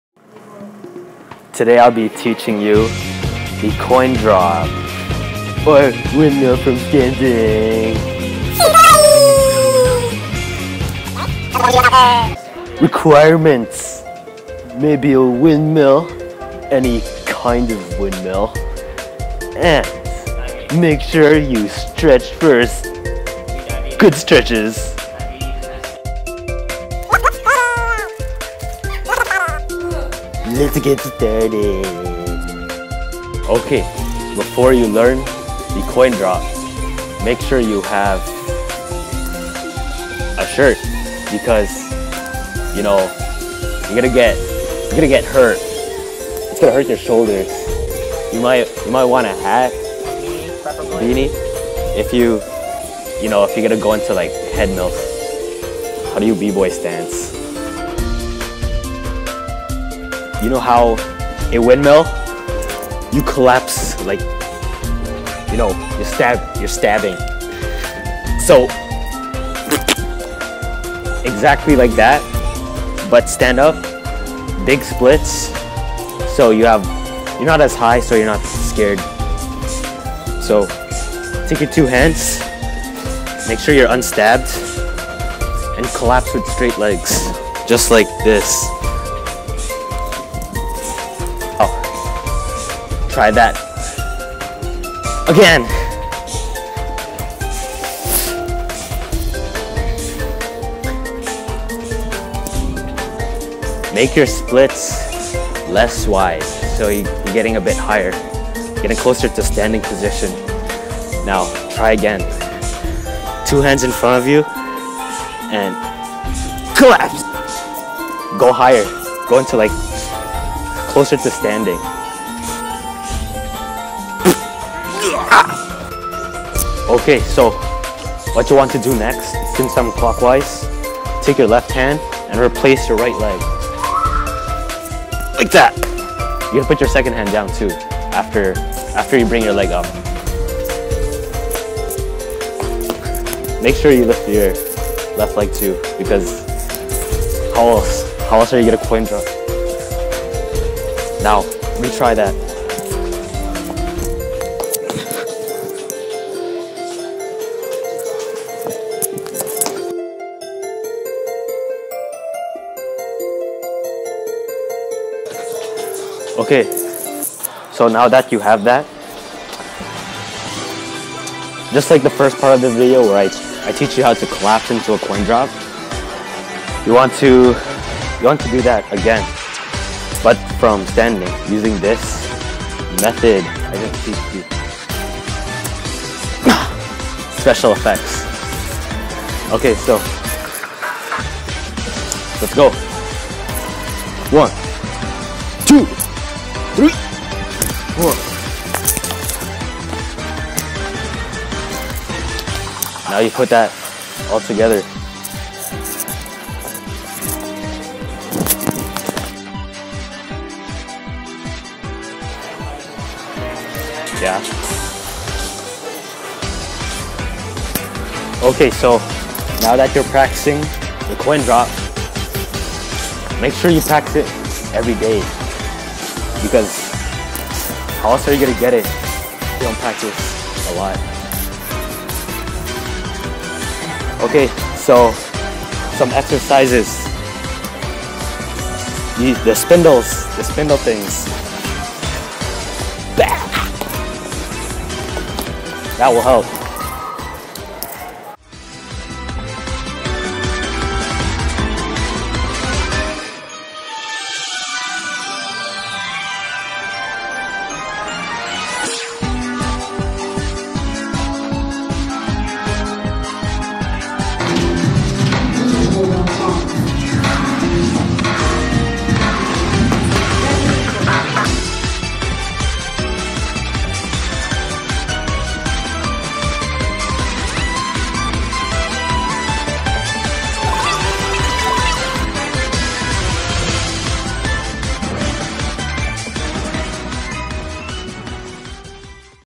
Breakdance: How to Coindrop Tutorial (CW)
Windmill from standing. I have this method of learning it is easier.

coindrop coin drop tut elingo rail gun bboy break dance clock wise CW clockwise how to powermove power move